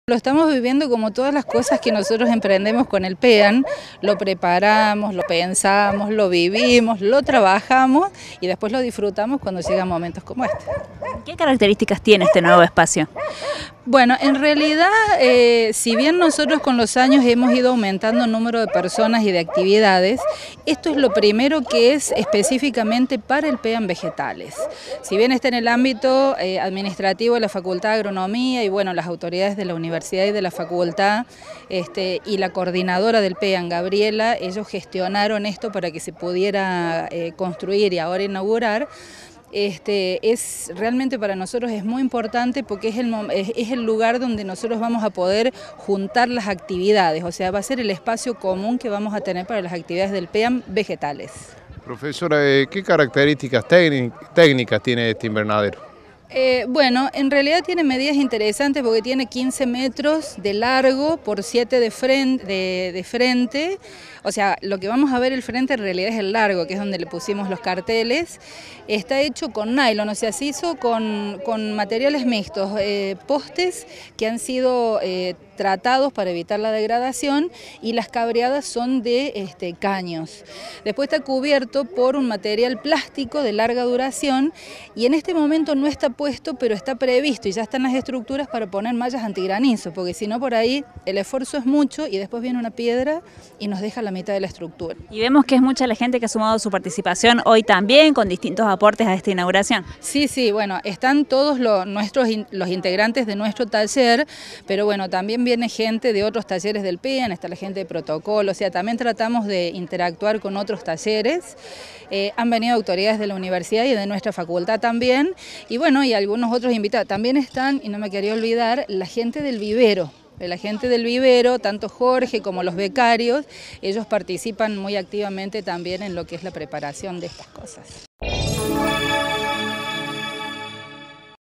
INVERNADERO DE LA UNRC - SUSANA VIALE – DOCENTE UNRC
LA INICIATIVA FUE IMPULSADA POR TALLERES DEL PEAM
Facebook.com/uniriotv
Twitter.com/uniriotv